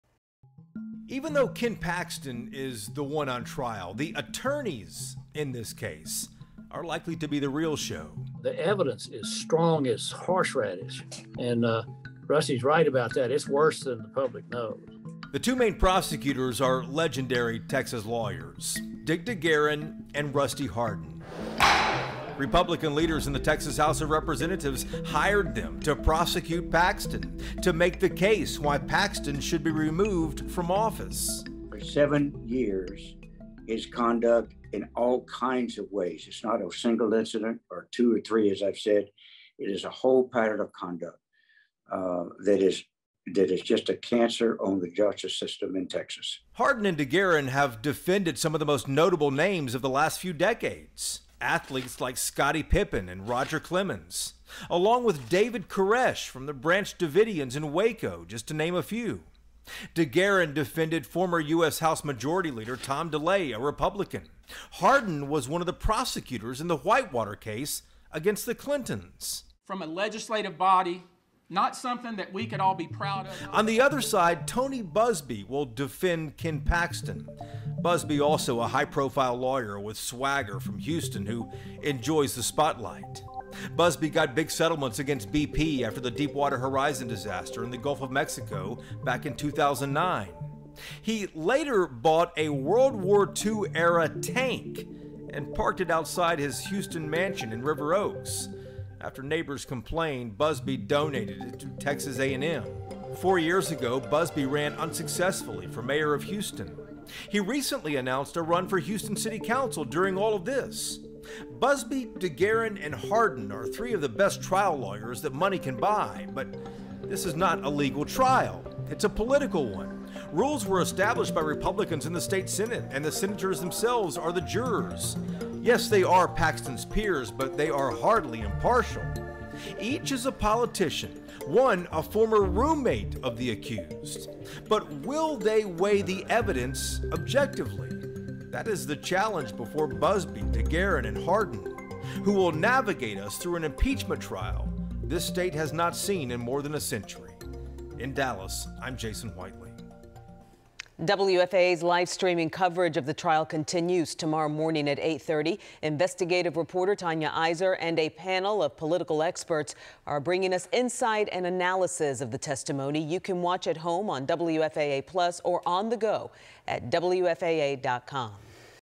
Who are the attorneys in the Paxton impeachment hearing?
The two main prosecutors are legendary Texas lawyers and Paxton's defense is running for Houston City Council.